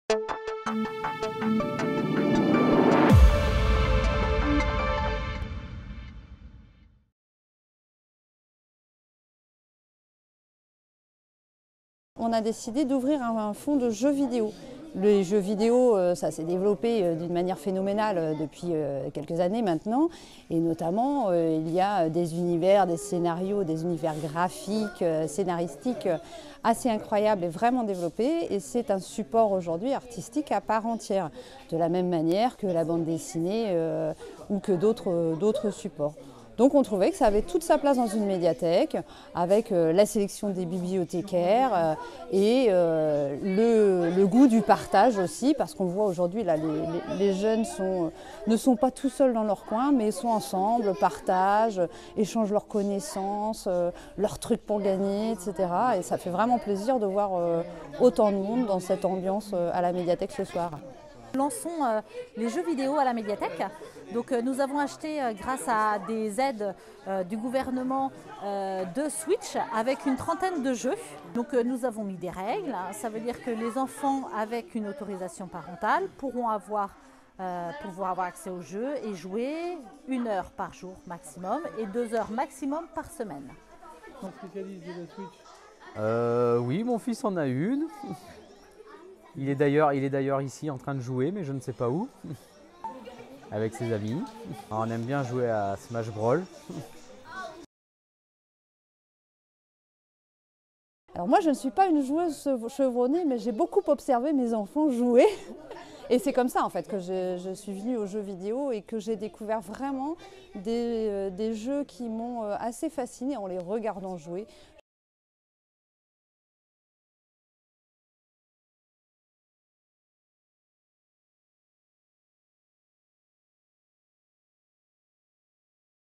2022-03-18 Animation numérique à la médiathèque
Vendredi 18 mars, la médiathèque de Saint -Aubin-du-Cormier a organisé une soirée " jeux vidéos". La médiathèque a acheté deux "Switch" ainsi que 30 jeux Ces équipements seront à disposition de chacun sauf le jeudi , journée sans écran.